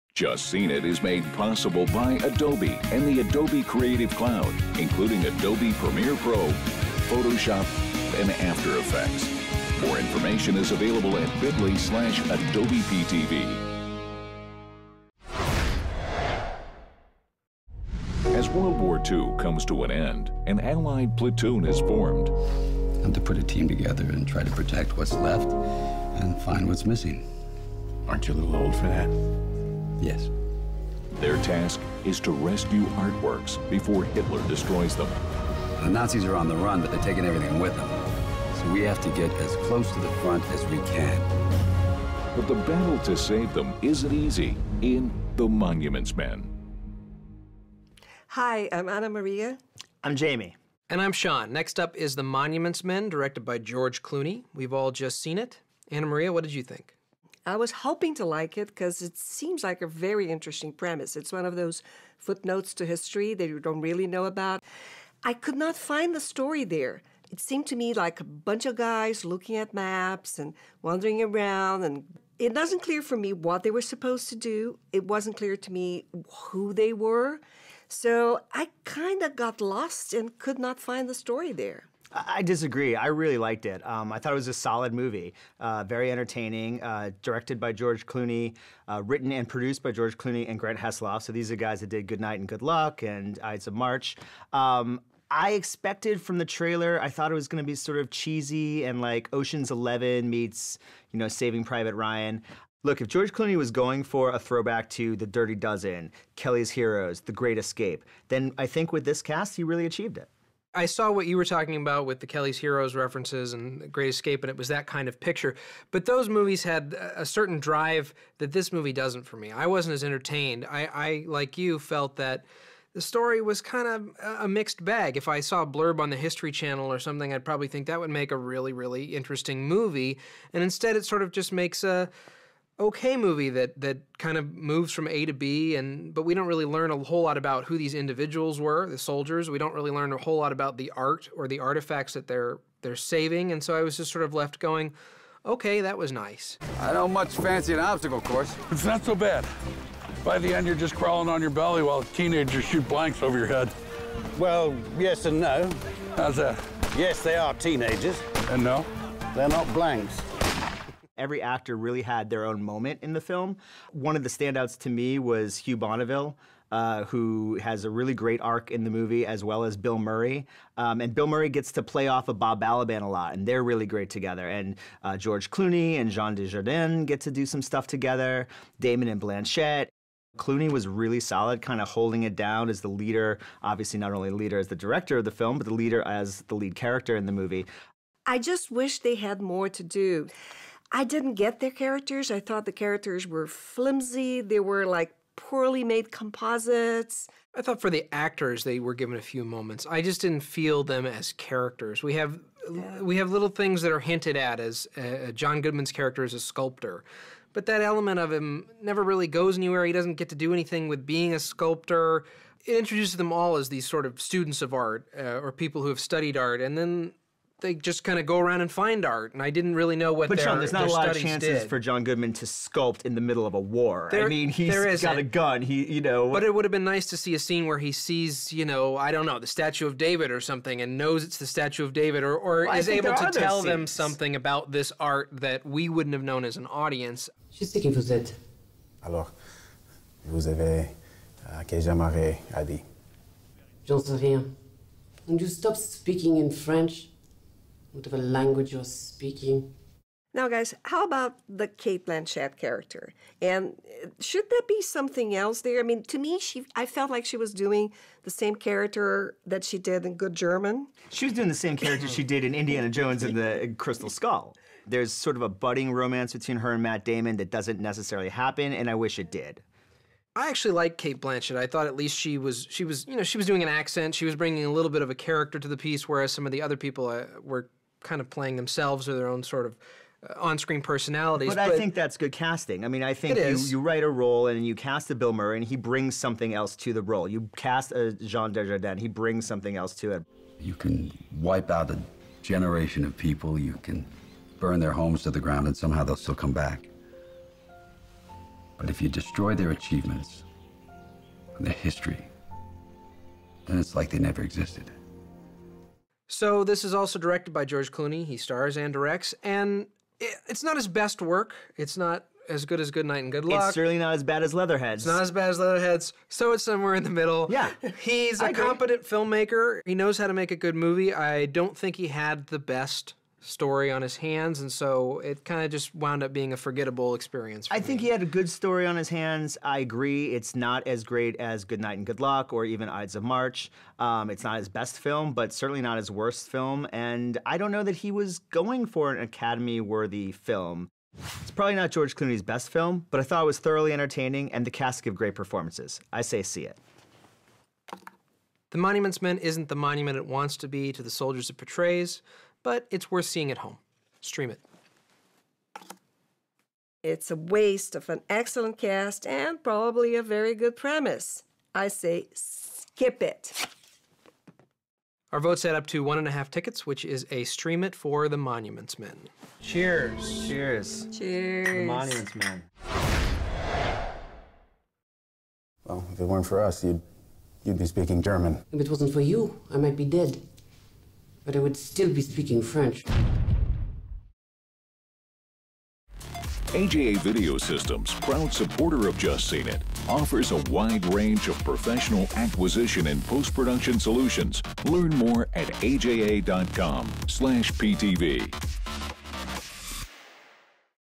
The Monuments Men Movie Review- Just Seen It
As World War II comes to an end, an Allied platoon is formed. Their task is to rescue artworks before Hitler destroys them. But the battle to save them isn't easy.

Starring George Clooney, Matt Damon, Bill Murray, and Cate Blanchett.
Directed by George Clooney.
Written by George Clooney and Grant Heslov.
Produced by George Clooney and Grant Heslov.
Genre: Action, Drama, Biographical, War

Guest reviewers Jami and Ana Maria join Sean as they review the new World War Two drama.

Starring Jami Philbrick, Ana Maria Bahiana, and Sean Wright.
Directed by Erik Howell.
Edited by Stephen Krystek.
Sound Design by Aaron Fink and Nick Isaacs.
Produced by David Freedman, Cooper Griggs,  Aaron Fink, Pedro Raposo, Amy Taylor, Liz Manashil and Hannah Wade.